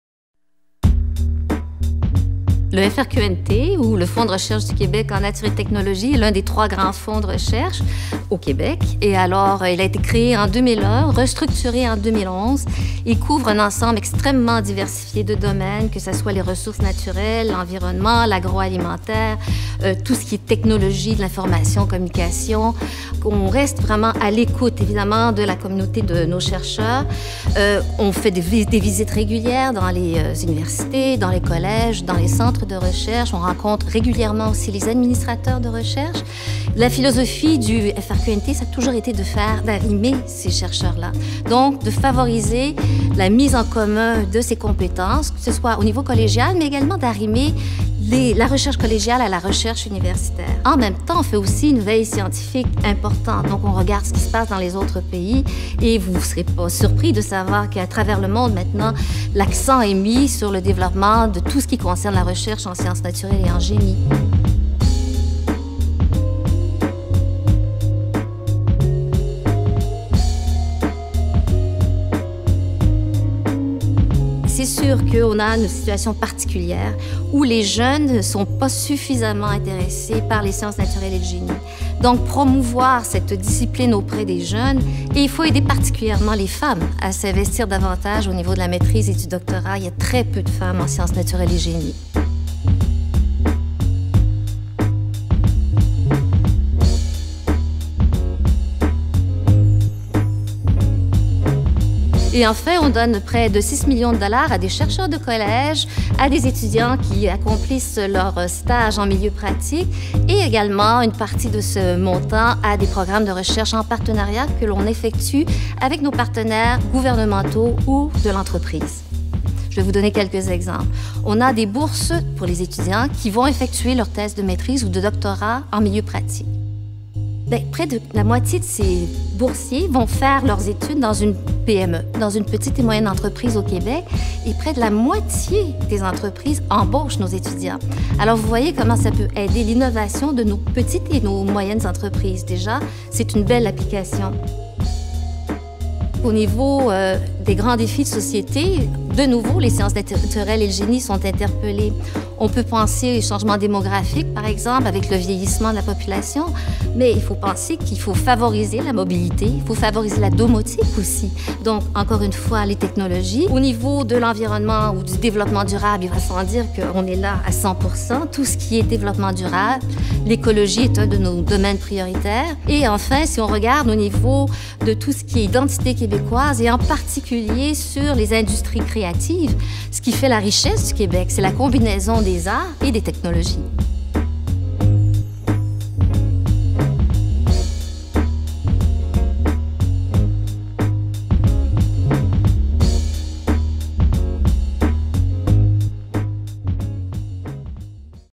| FRQNT | Présentation du Fonds de recherche du Québec - Nature et technologies | Maryse Lassonde
La directrice scientifique du Fonds de recherche du Québec - Nature et technologies (FRQNT), Maryse Lassonde, présente le Fonds : sa mission et ses orientations stratégiques, ainsi que ses programmes.

Date d'enregistrement: 2 décembre 2013